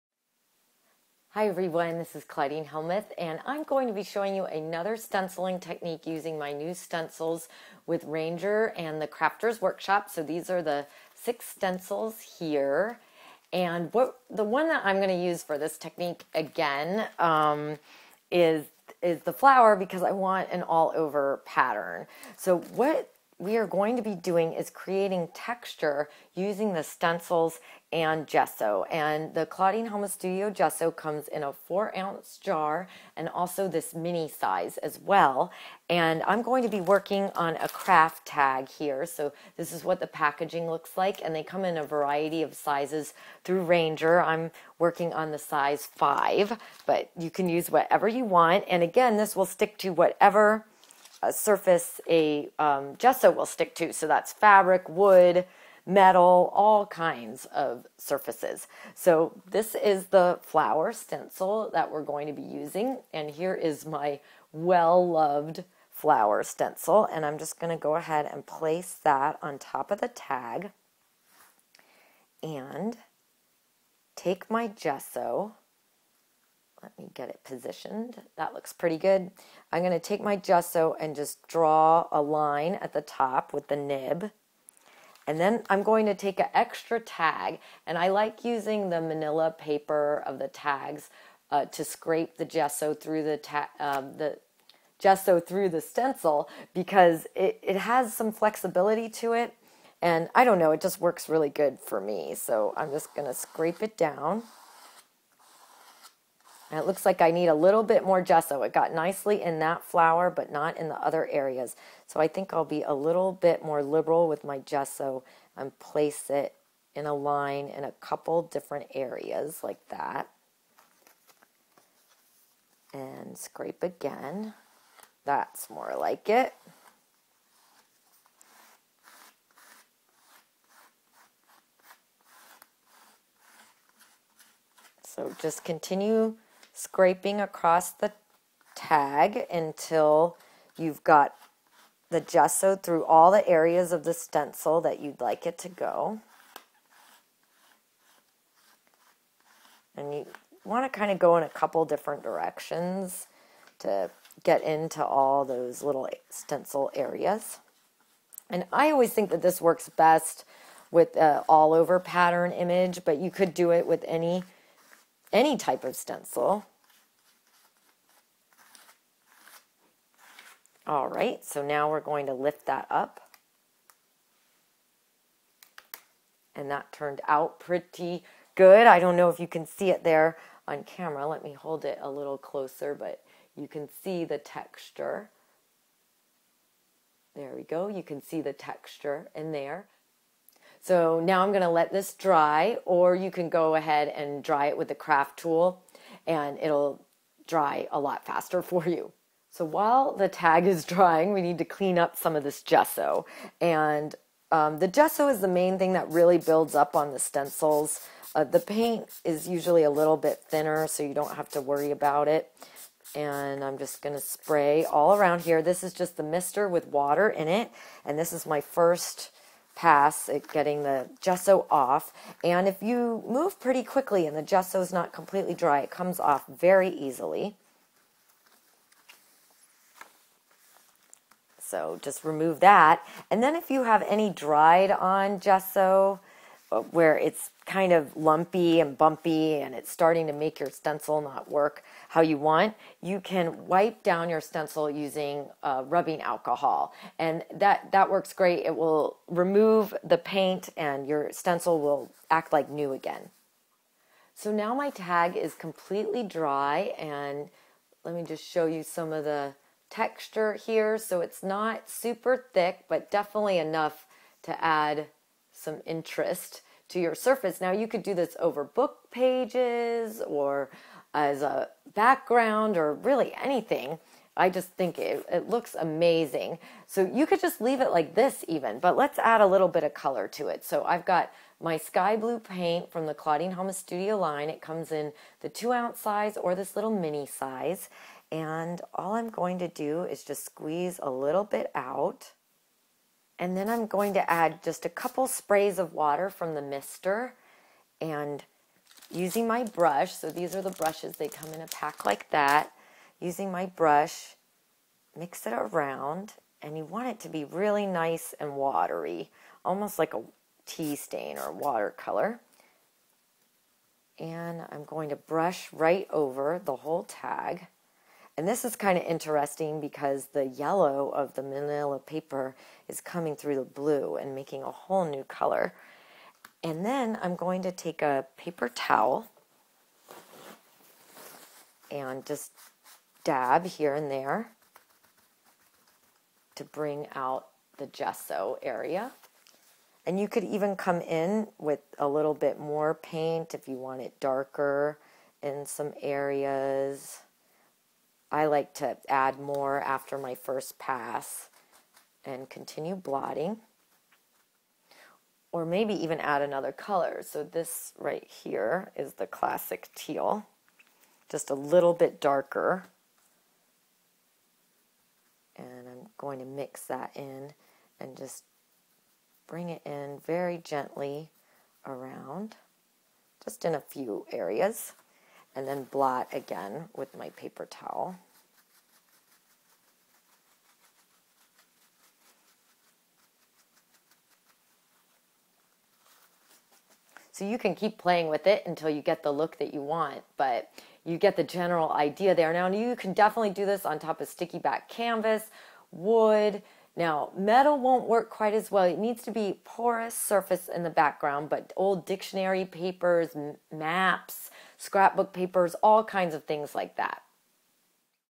gesso texture stencil technique
gesso texture stencil technique using Claudine Hellmuth studio stencils and gesso